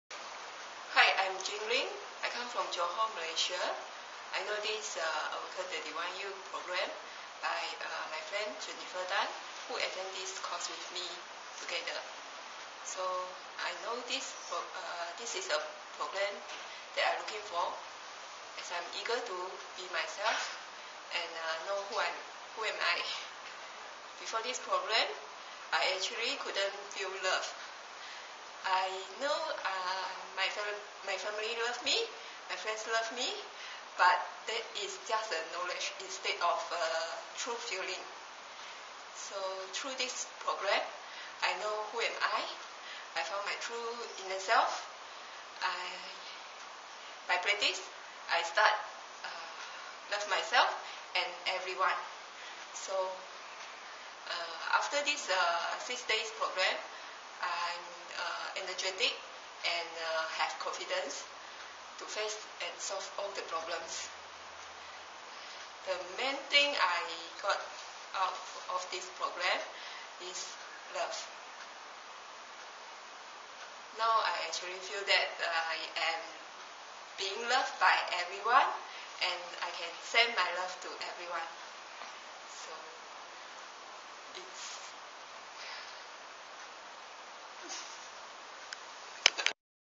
"Awaken~The Divine You" ® Programme Foundation to Personal Mastery July '12 Testimony Moo Jing Ling
These student testimonials are from"Awaken~The Divine You" ® (Non-Religious & Universal) 6 day Foundation Level To Personal Mastery Workshop held in Malaysia in July 2012 conducted by THE GOLDEN SPACE CENTRE.